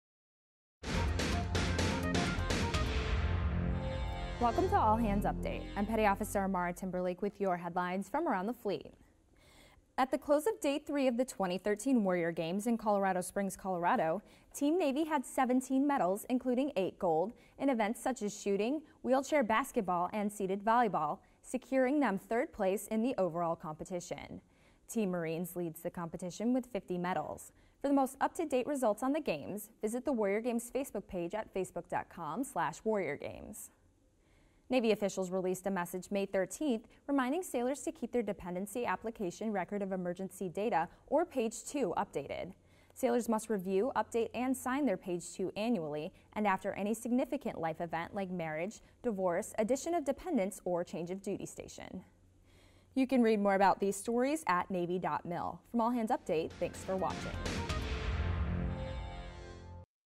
Warrior Games Team Navy Recap; Navy Reminds Sailors to Update Page Two
Headlines for Wednesday, May 15, 2013: Warrior Games Team Navy Recap; Navy Reminds Sailors to Update Page Two